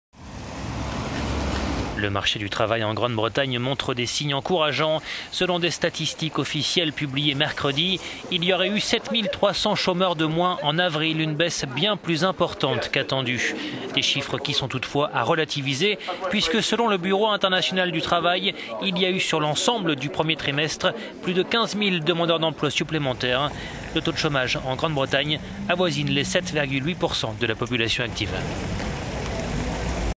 Grande-Bretagne : le chômage a reculé en avril - economy
Le marché du travail en Grande-Bretagne montre des signes encourageants. Selon des statistiques officielles publiées mercredi, il y aurait eu 7300 chômeurs de moins en avril, une baisse bien plus importante qu'attendue.

Des chiffres qui sont toutefois à relativiser, puisque selon le Bureau international du travail, il y a eu, sur l'ensemble du premier trimestre, plus que 15 000 demandeurs d'emplois supplémentaires. Le Taux de chômage en Grande-Bretagne avoisine les 7,8% de la population active.